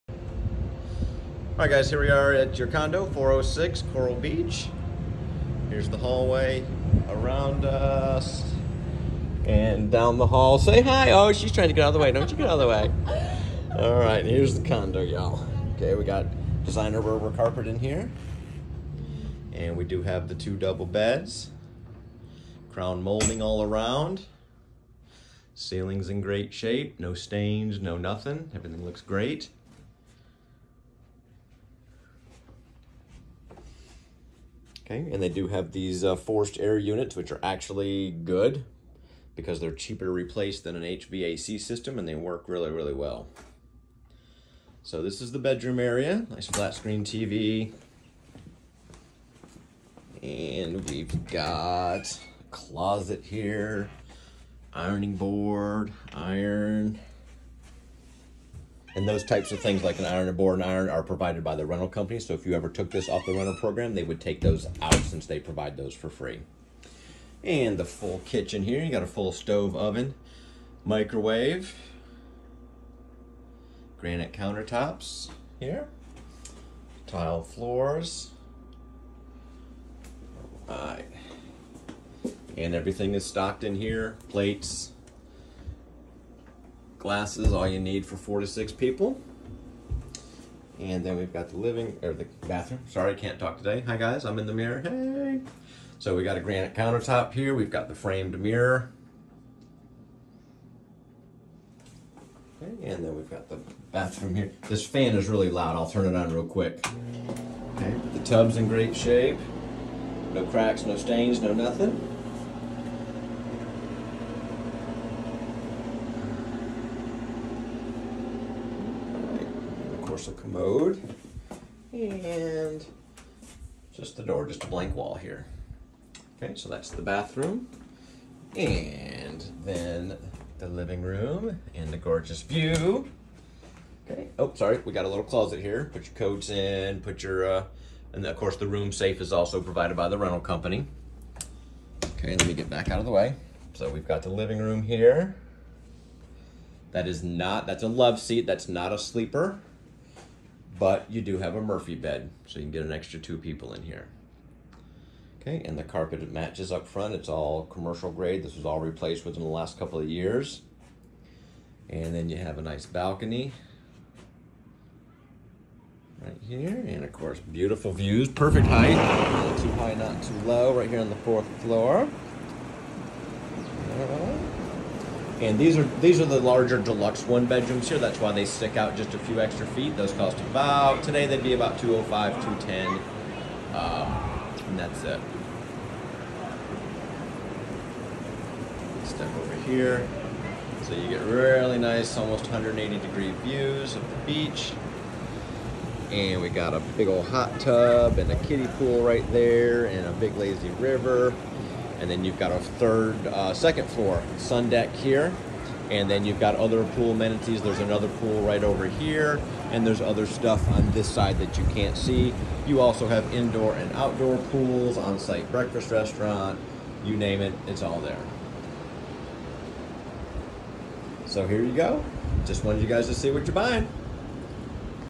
#406 Coral Beach Resort
Direct oceanfront 1 BR / 1 BA furnished and turn-key condo in  Myrtle Beach, SC.